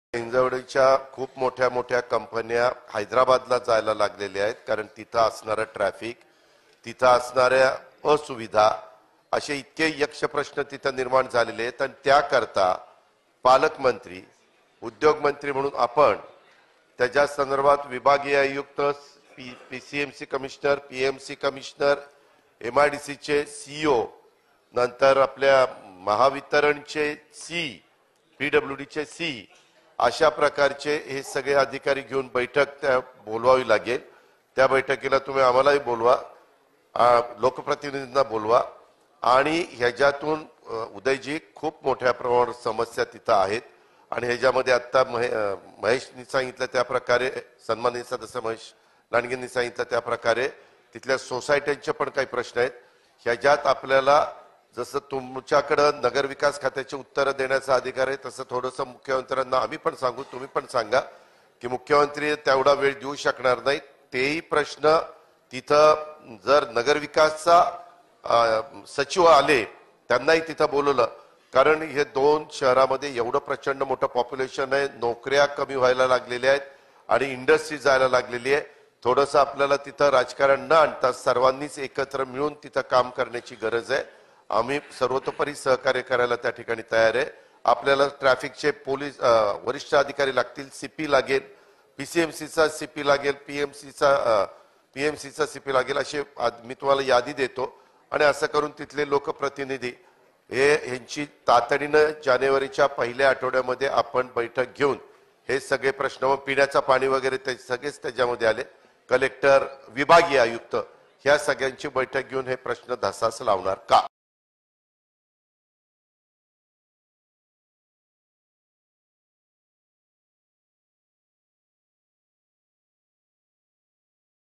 Ajit Pawar on Pune Traffic: 'ट्रॅफिकमुळे कंपन्या बाकी राज्यांत गेल्या'; अजित पवारांनी मांडला मुद्दा
पुणे आणि पिंपरी-चिंचवड शहरातील वाहतूक, पिण्याच्या पाण्याचा प्रश्न आज विधानसभेत विरोधी पक्षनेते अजित पवारांनी उपस्थित केला. 'पुण्यातील ट्रॅफिकमुळे कंपन्या बाकी राज्यांत गेल्या' असे विधान करत लवकरात लवकर या विषयी तोडगा काढावा' असे आवाहन राज्य सरकारला केले.

Get exclusive coverage of Himachal Pradesh and Gujarat Assembly Election results 2022 in English, Hindi and Gujarati